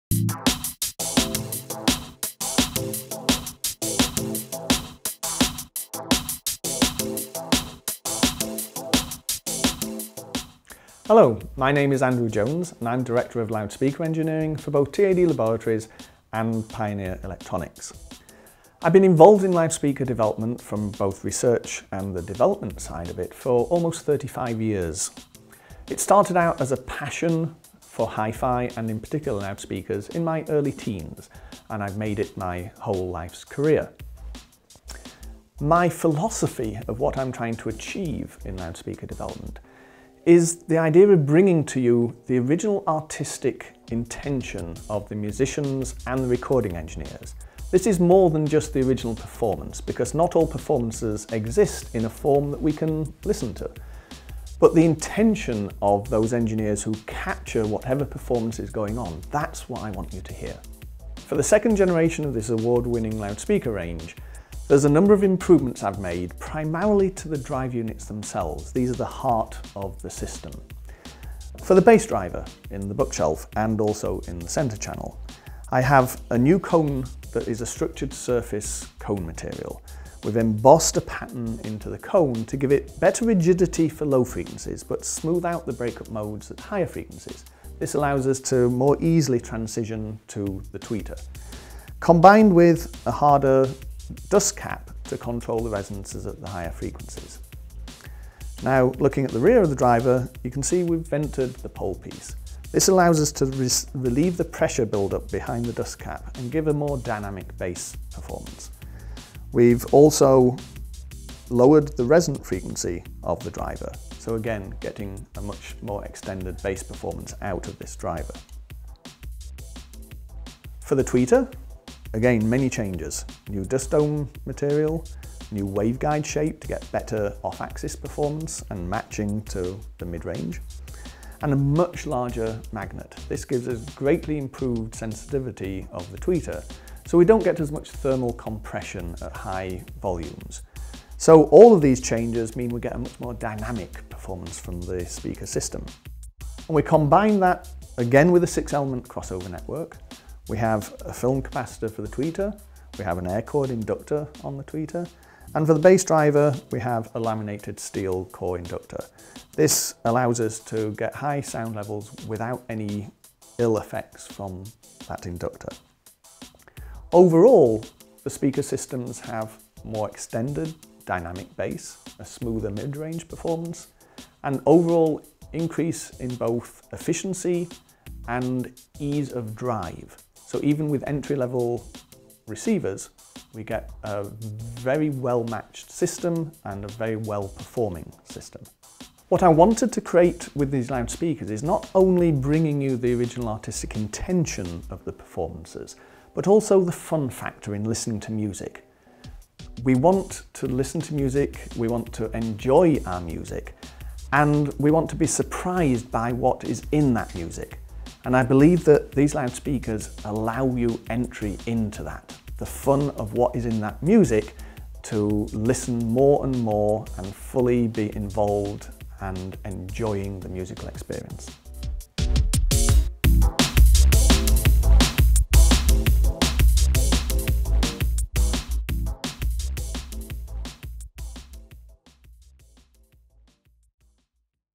Andrew Jones Speaker Design Philosophy
Pioneer's Chief Speaker Engineer, Andrew Jones talks about what his goals are when he designs, develops, and builds home theater speakers.